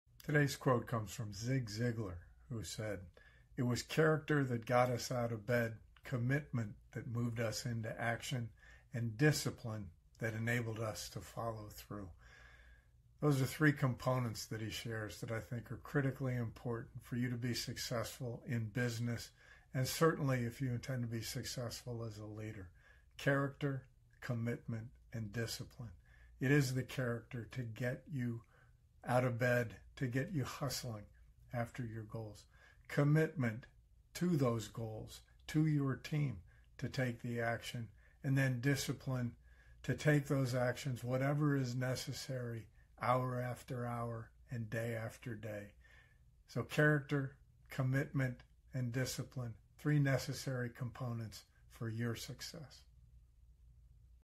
Qoute of the Day - Zig Ziglar
Do you know the differences between the roles that character, commitment and discipline play in achieving success?